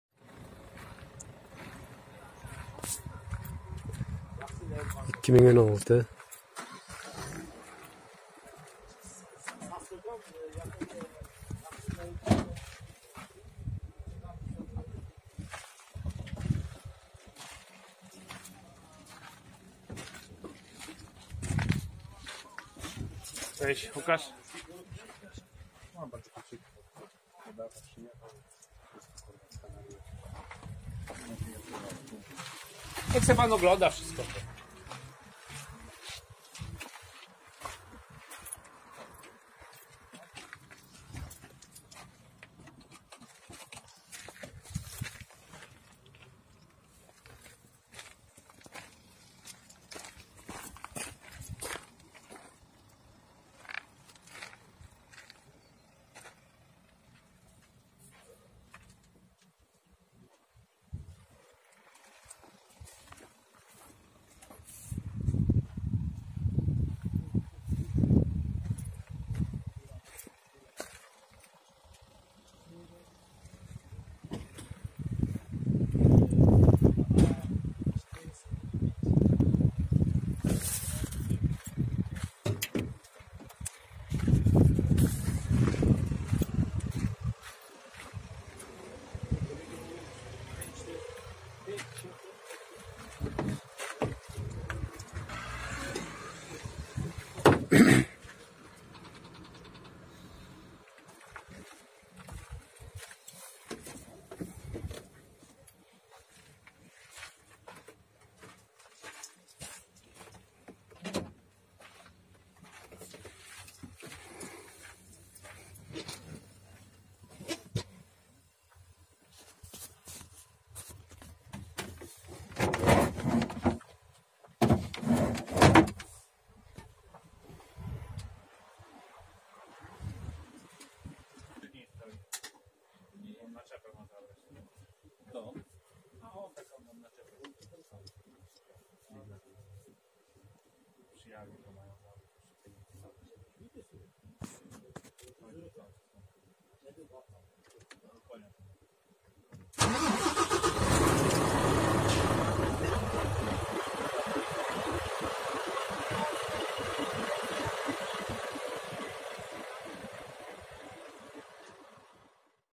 VOLVO 500, стандарт 2016. Автомат Евро 6
ВОЛВО 500, стандарт
🗓. 2016.04
👣 678 000 км
🕹  Автомат
🔧 Без дефект
🇪🇺 Евро 6
💶 47 000 €  Беларусгача